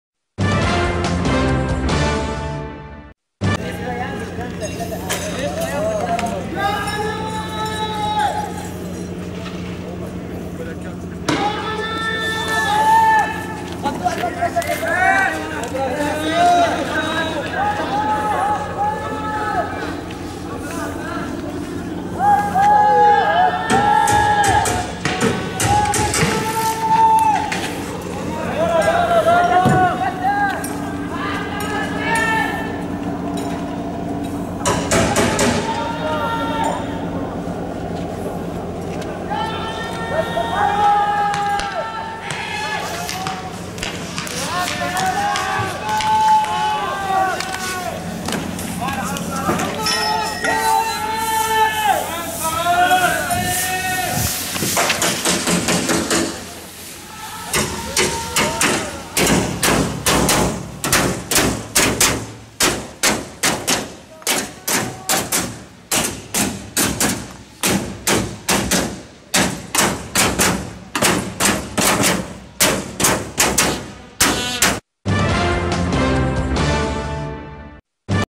إستهداف المتظاهرين بالطلق المباشر بالسنابس 3/12/2013 Bahrain
إستهدفت قوات النظام المتظاهرين مساء يوم الثلاثاء (3 ديسمبر/كانون الثاني 2013) في بمنطقة السنابس غرب العاصمة البحرينية المنامة بسلاح C4 بعد قمع تظاهرة تنديداً بمقتل الشهيد أحمد عبدالأمير "15 عاما" الذي قضى متأثراً بإصابته بحروق بليغة